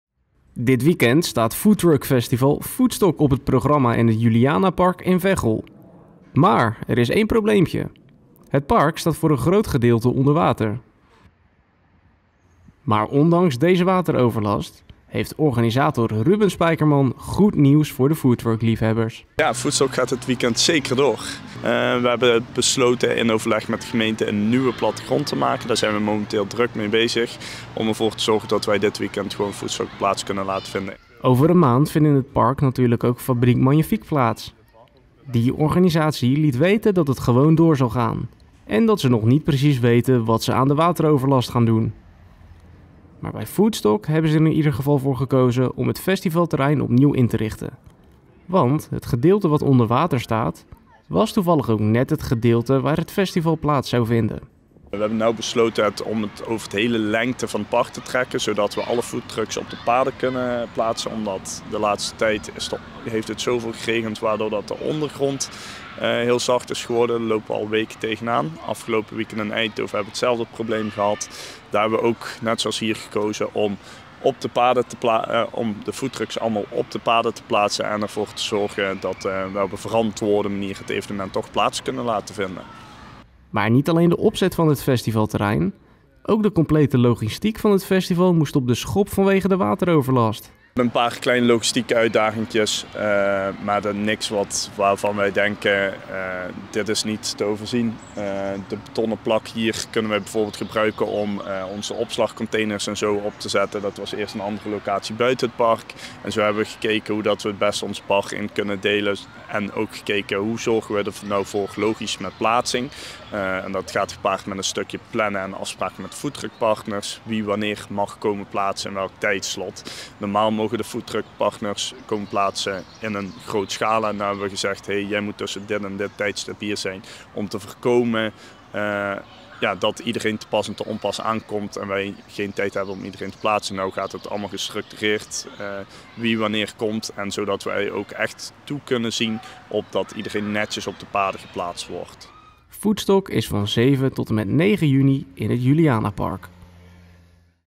Foodstock Veghel op de schop door wateroverlast I Omroep Meierij
Dit weekend staat foodtruck-festival foodstock op het programma in het Julianapark in Veghel, maar er is één probleempje; het park staat voor een groot gedeelte onder water. Maar ondanks deze wateroverlast heeft organisator Ruben Spijkerman goed nieuws voor de foodtruck-liefhebbers.

Kijk voor meer artikelen op omroepmeierij.nl